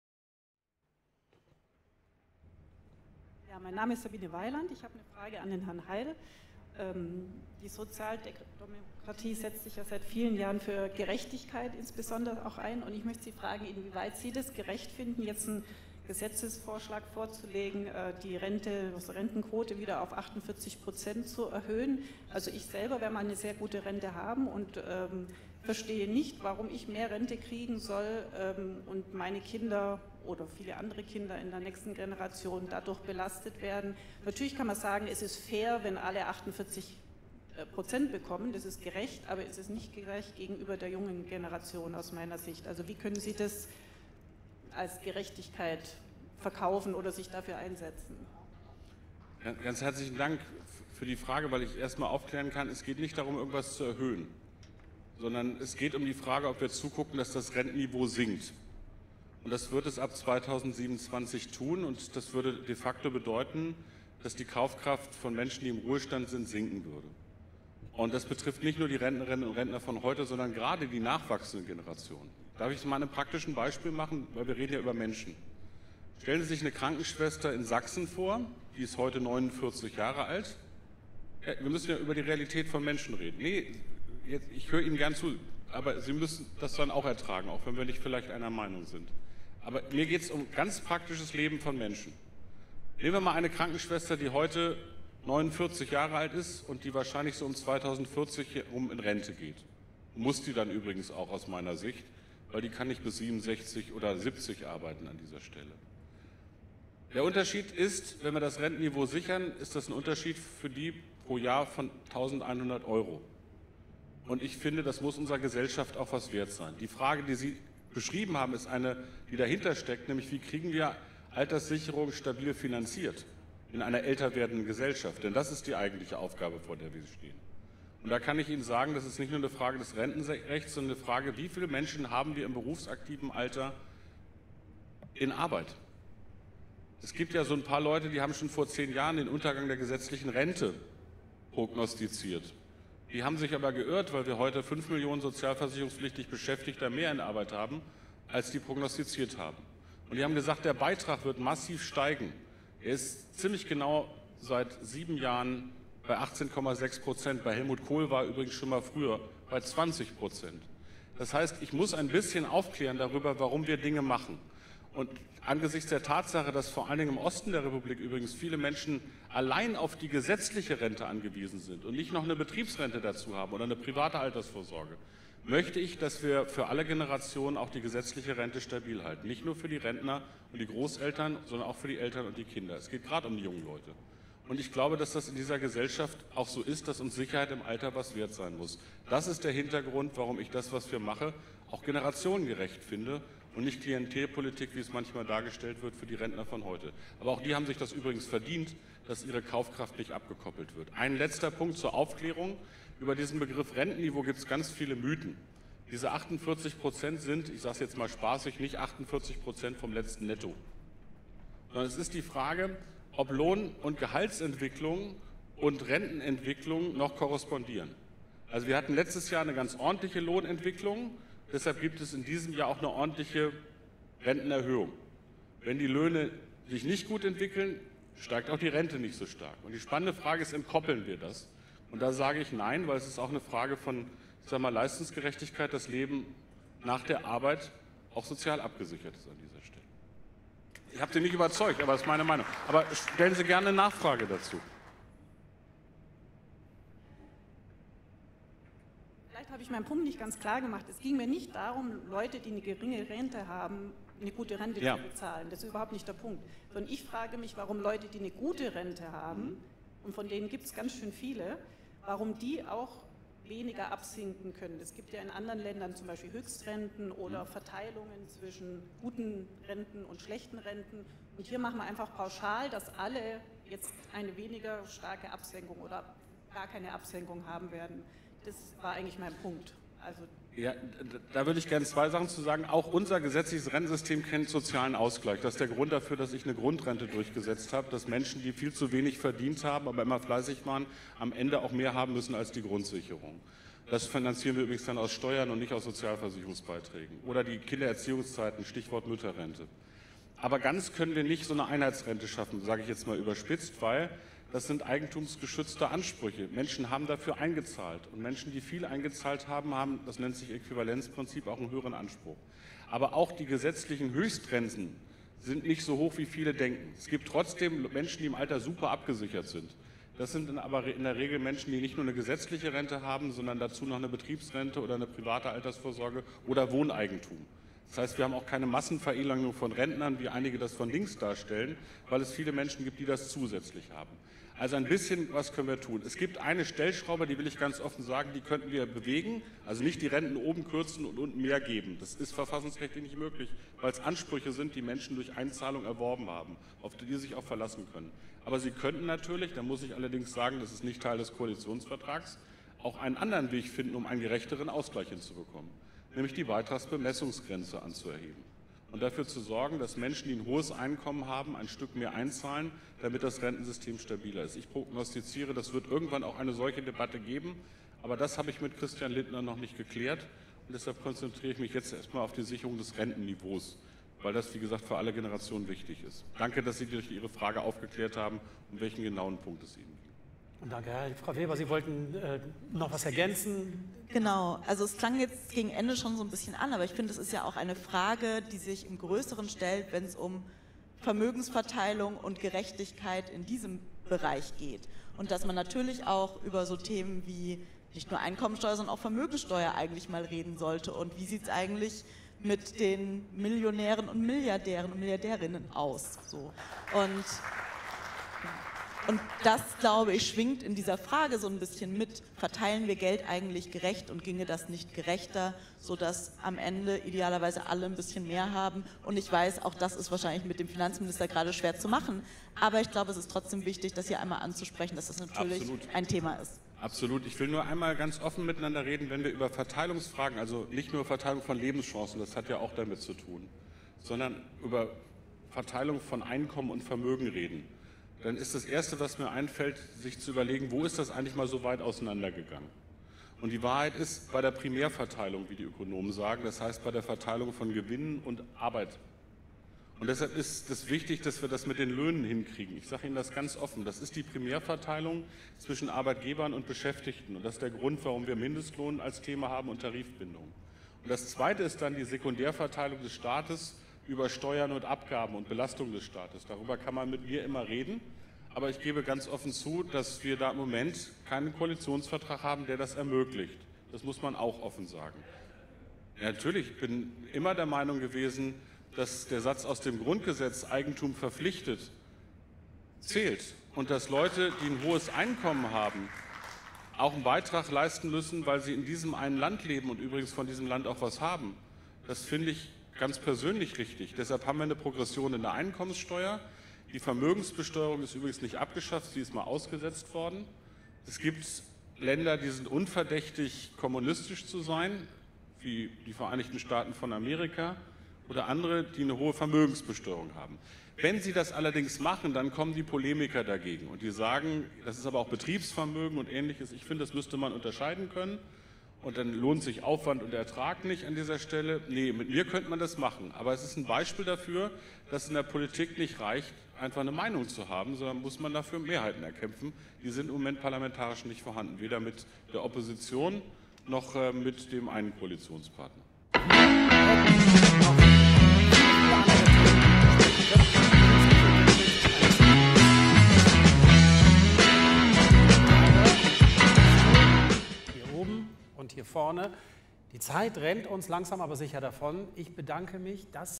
Hubertus Heil zur Rente auf dem Demokratiefest in Berlin
Wie sieht die Rente in Zukunft aus? Der Bundesminister für Arbeit und Soziales, Hubertus Heil (SPD) musste sich auf dem Demokratiefest einer kritischen Frage stellen.

Die Antwort überrascht nicht, aber zwischen den Zeilen lässt sich ein wenig deuten. Behindert die FDP am Ende einige Rentenpläne des Ministers?